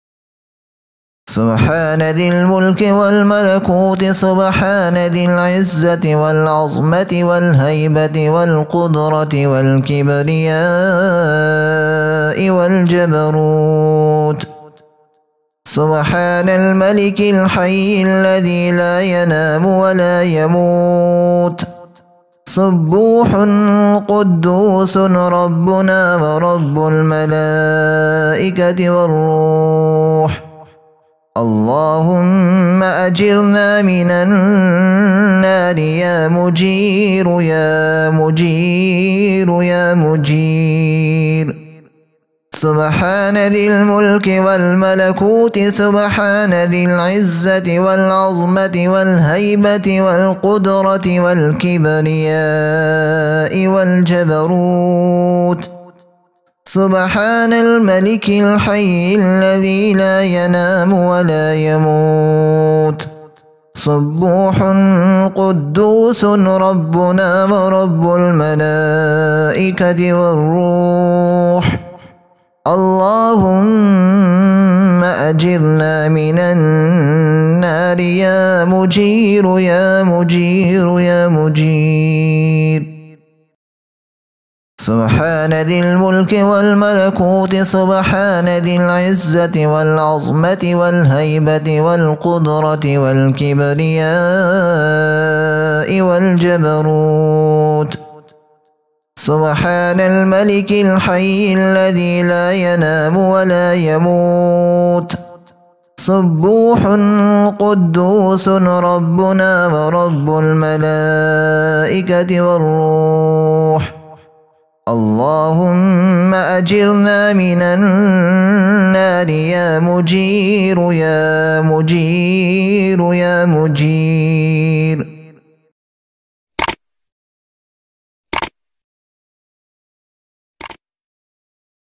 Dua Taraweeh | Tasbeeh Taraweeh | Taraweeh ki Dua
Tasbeeh e Taraweeh Full with arabic text. Dua e Taraweeh repeat 3 times.